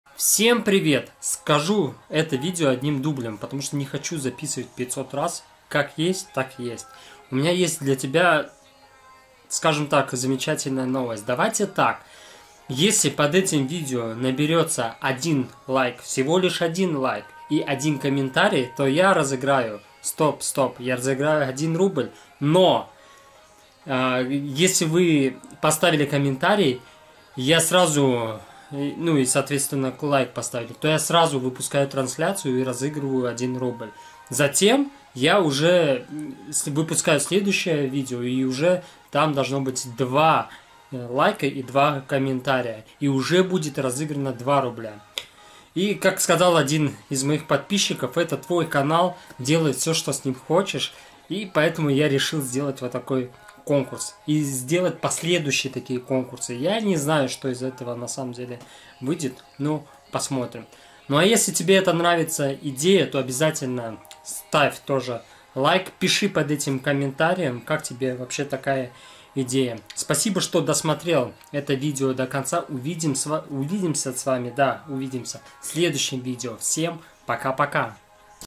Конкурс, выиграй 1 рубль челлендж.Участвуй в этом конкурсе и победи.
Всем привет, правило конкурса, челленджа, как хотите так и называйте, быть подписанным на канал, поставить лайк и оставить всего один комментарий, победителя выберем в прямой трансляции и важно победителю быть на прямой трансляции.Нет на трансляции, деньги уходят в банк в следующую трансляцию.